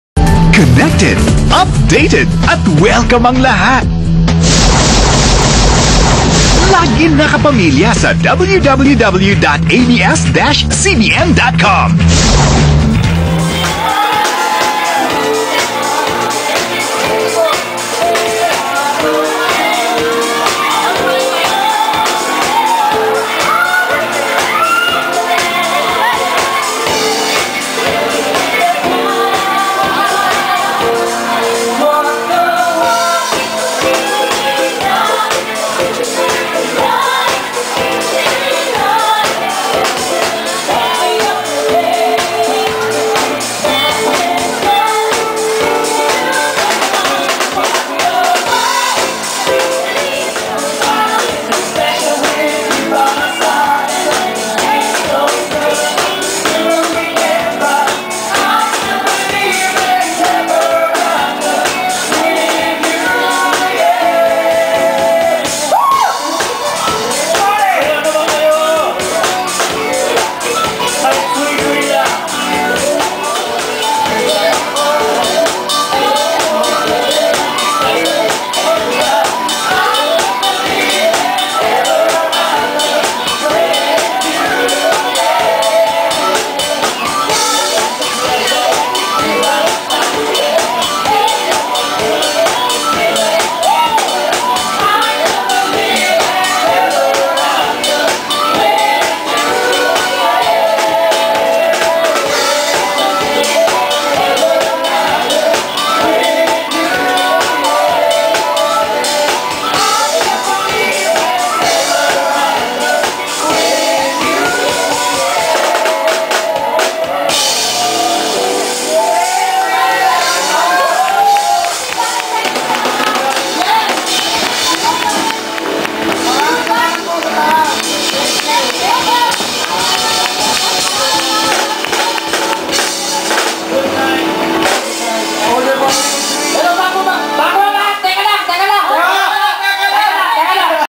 Status Concert 3-3
Status: A Valentine's Night with The Scholars.  February 13, 2009 at Ratsky's Tomas Morato.  Featuring PDA2 ex-scholars Chivas, Bunny, Poy, Cris, Liezel, Miguel, and Bugoy with Ruben Gonzaga.  (part 3 of 3)